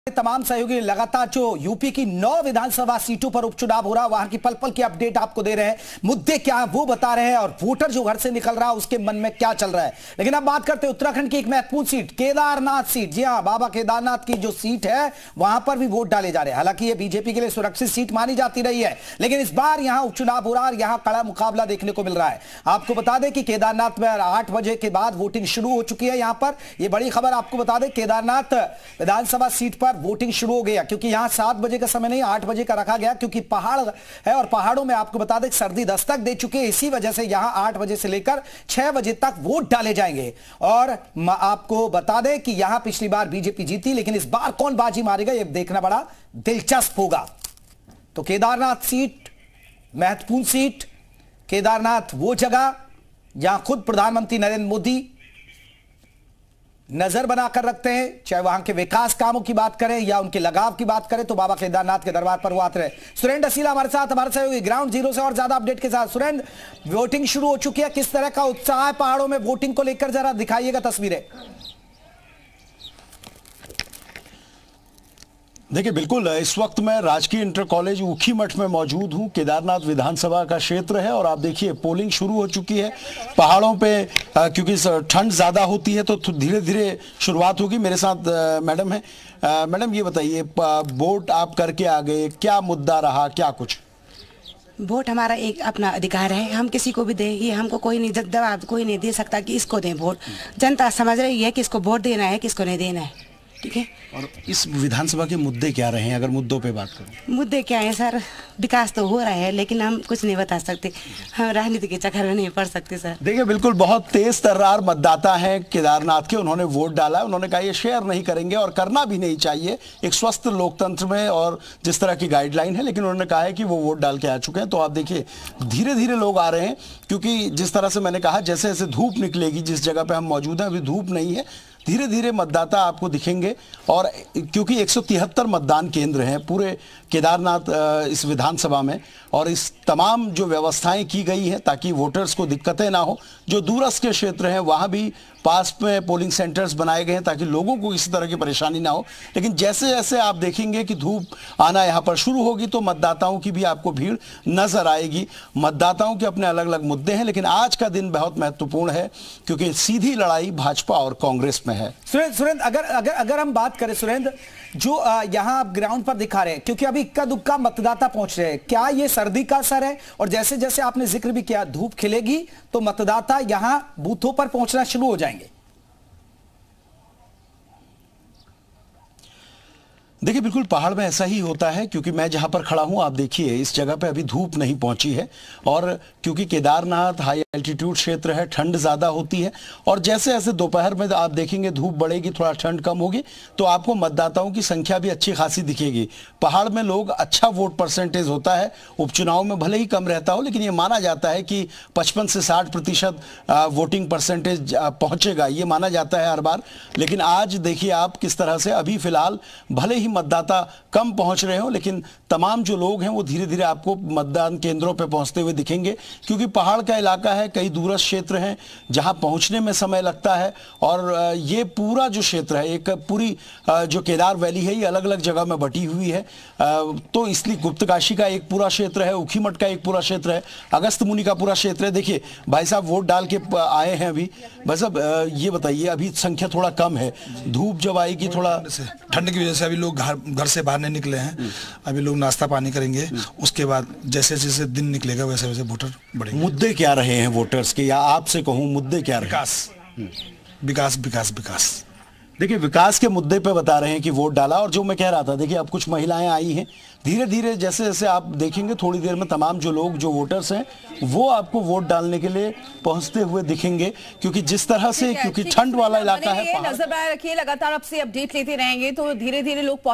Kedarnath By Election 2024 : केदारनाथ में वोटिंग जारी, 23 नवंबर को रिजल्ट आएगा,| BJP | Congress
KedarnathByElection: केदारनाथ में वोटिंग जारी, 23 नवंबर को रिजल्ट आएगा, बीजेपी से आशा नौटियाल प्रत्याशी, कांग्रेस से मनोज रावत प्रत्याशी

KedarnathByElection: Voting continues in Kedarnath, result will come on November 23, Asha Nautiyal candidate from BJP, Manoj Rawat candidate from Congress.